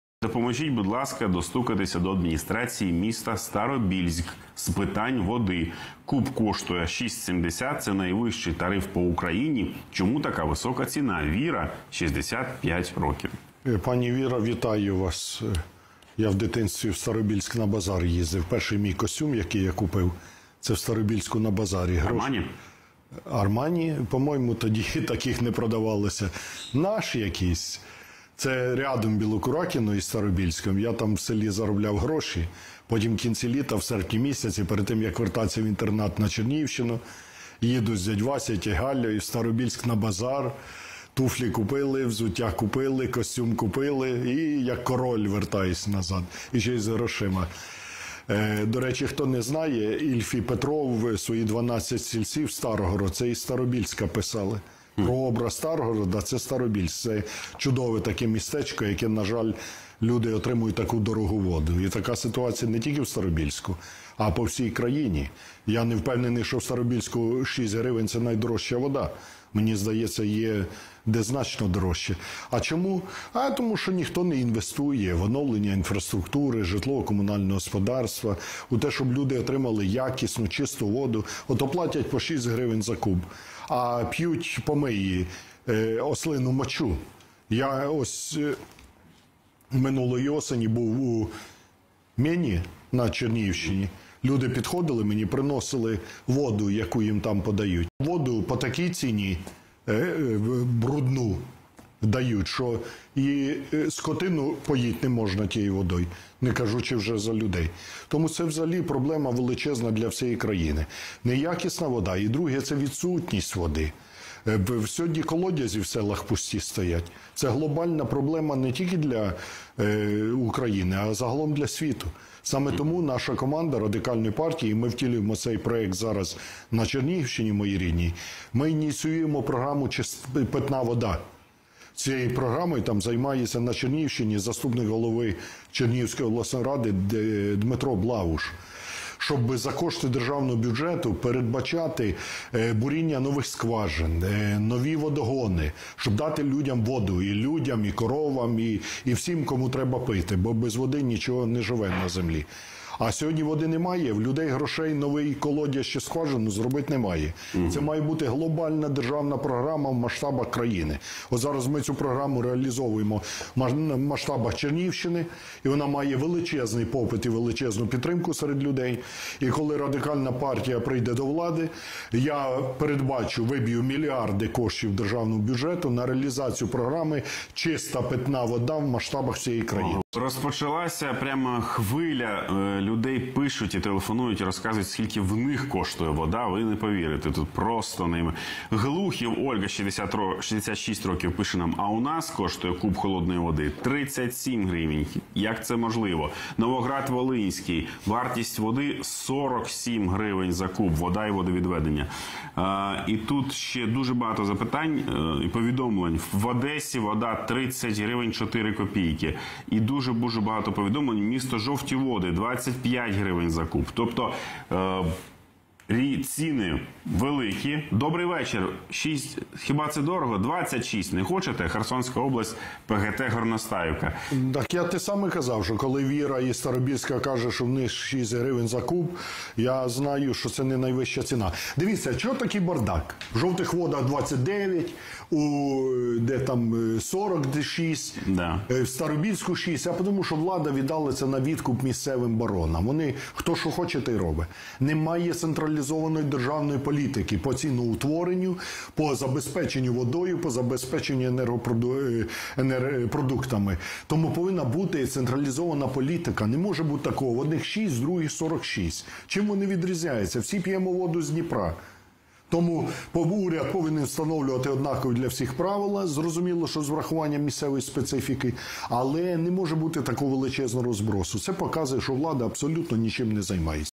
Ляшко: Не повинно такого бути, щоб одні платили за куб води 6 грн, а інші 46
Впровадити масштабну програму «Питна вода» в межах всієї країни вимагає від влади Олег Ляшко. Про це лідер Радикальної партії заявив в ефірі телеканалу «Україна 24», відповідаючи на запитання глядачів. Так, 65-річна Віра зі Старобільська пожалілася на неякісну питну воду та зависоку ціну на неї. Політик підкреслив: така проблема турбує людей по всій країні.
«Неякісна вода і її відсутність – дві головні проблеми в масштабах всієї країни. Саме тому на моїй рідній Чернігівщині ми ініціювали програму «Питна вода», якою займається заступник голови Обласної ради від Радикальної партії Дмитро Блауш. За кошти державного бюджету передбачається буріння нових свердловин, встановлення нових водогонів, щоб дати людям якісну питну воду, бо сьогодні у українців немає грошей на нові колодязі та все інше. Це має бути глобальна всеукраїнська програма. Коли Радикальна партія прийде до влади, я виб’ю мільярди гривень з державного бюджету на цю програму», - заявив Ляшко.
Після відповіді політика, в ефірі почалась хвиля повідомлень від глядачів з різних куточків країни щодо цін на воду. Різниця шокуюча – від 6 гривень за куб води у Старобільську до 47 гривень за куб води у Новоград-Волинському.
«Чому такий бардак? Тому що влада віддала це на відкуп місцевим «баронам». А вони – хто що хоче, те й і робить. Немає централізованої державної політики з ціноутворення та забезпечення водою. А такого не повинно бути: у одних 6 гривень за куб води, а в інших – 46. Всі п’ємо воду з Дніпра. Це показує, що влада абсолютно нічим не займається. Тому уряд повинен встановлювати однакові для всіх правила з урахуванням місцевої специфіки. При цьому, не може бути такої величезної розбіжності», - наголосив Ляшко.